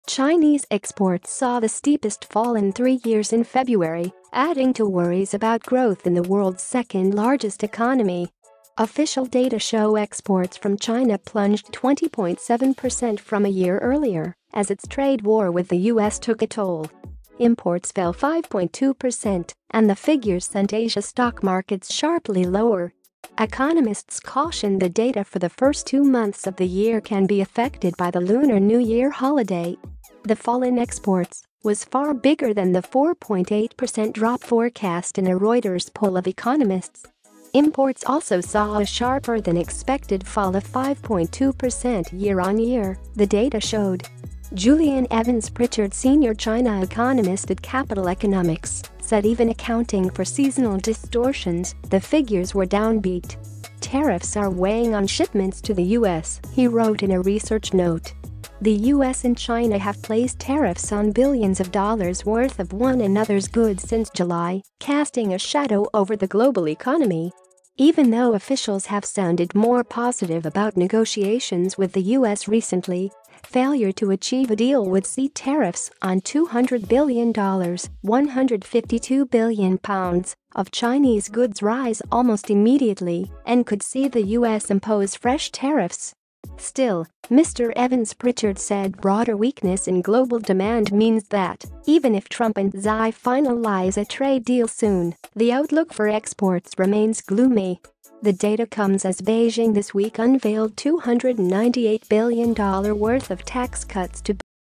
China exports slump 21% in February
Chinese exports saw the steepest fall in three years in February, adding to worries about growth in the world's second largest economy. Official data show exports from China plunged 20.7% from a year earlier, as its trade war with the US took a toll. Imports fell 5.2% and the figures sent Asia stock markets sharply lower. Economists caution the data for the first two months of the year can be affected by the Lunar New Year holiday. The fall in exports was far bigger than the 4.8% drop forecast in a Reuters poll of economists. Imports also saw a sharper than expected fall of 5.2% year-on-year, the data showed. Julian Evans-Pritchard, Senior China Economist at Capital Economics said even accounting for seasonal distortions, the figures were "downbeat". "Tariffs are weighing on shipments to the US," he wrote in a research note. The US and China have placed tariffs on billions of dollars worth of one another's goods since July, casting a shadow over the global economy. Even though officials have sounded more positive about negotiations with the US recently, failure to achieve a deal would see tariffs on $200bn (£152bn) of Chinese goods rise almost immediately and could see the US impose fresh tariffs. Still, Mr Evans-Pritchard said "broader weakness in global demand means that, even if Trump and Xi finalise a trade deal soon, the outlook for exports remains gloomy." The data comes as Beijing this week unveiled $298bn worth of tax cuts to boost slowing growth.